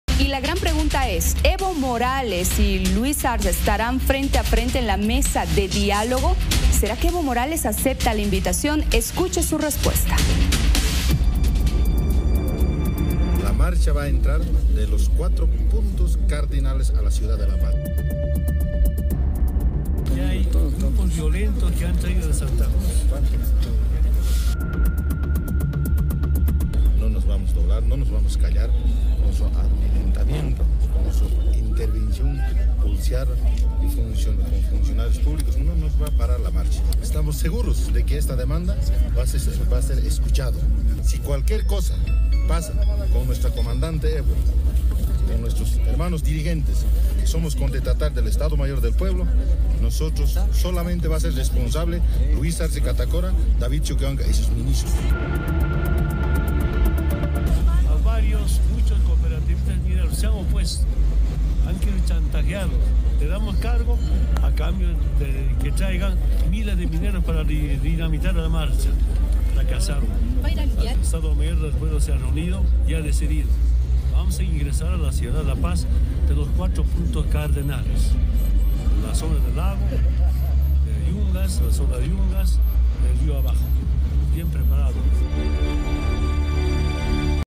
Cuarto día de marcha
Los manifestantes aseguran no haber recibido convocatoria al diálogo y continúan avanzando a sede de Gobierno.

Sectores afines a Evo Morales mencionan que no llegó ninguna convocatoria para el diálogo y anuncian arribo a La Paz por varias vías